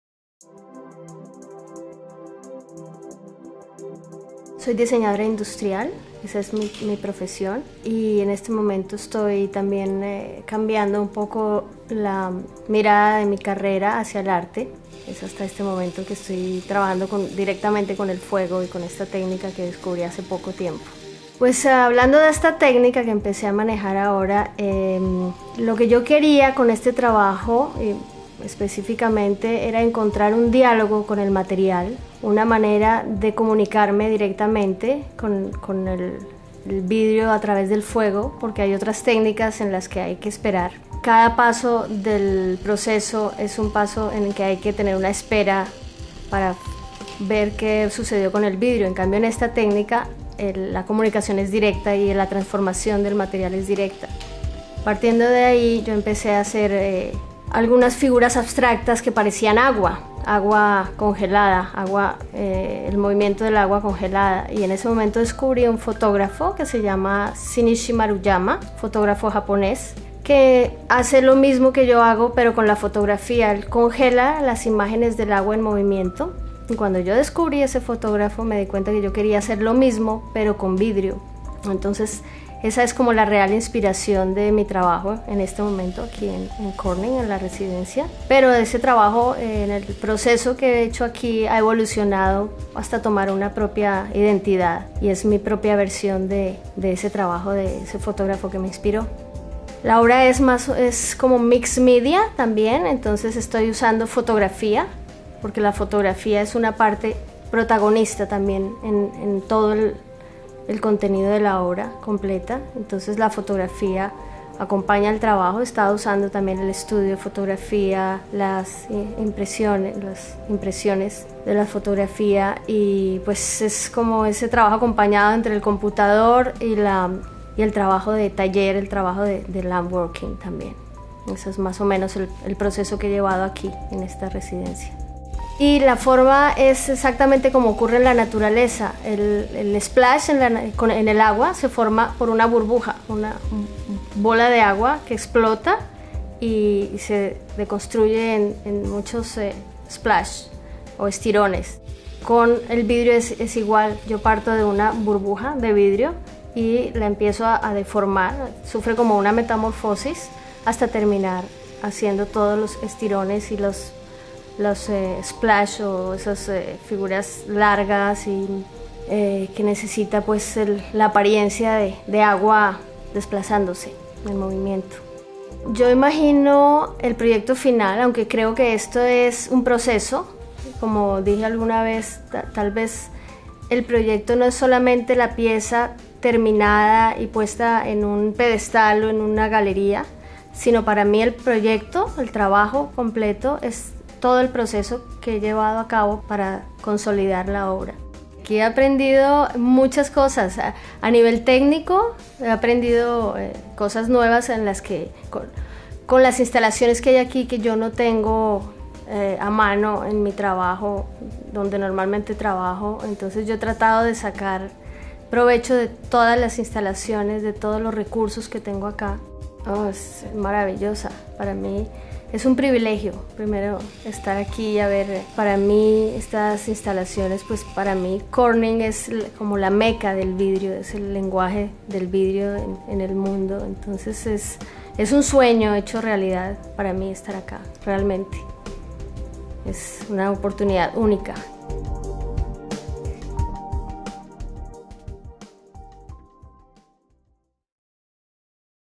Artist-in-Residence: Marta Ramírez
Marta Ramírez is a glass artist and industrial designer who teaches at the Los Andes University in Bogotá, Colombia. Her work is clearly inspired by water, and she explores the similarities of this element and the material of glass through her art. "Water is movement, transparency, gravity, freefall. Water curls and zigzags," says Ramírez. "Glass does too."

In her May 2012 Residency at The Studio, Ramírez will interpret the works of Shinichi Maruyama, a Japanese artist and photographer who has developed an artistic body of work called Water Sculptures. These works use the camera to capture frozen images of moving water. In Ramírez's residency, she will interpret the works of Maruyama, recreating the static moments of moving water.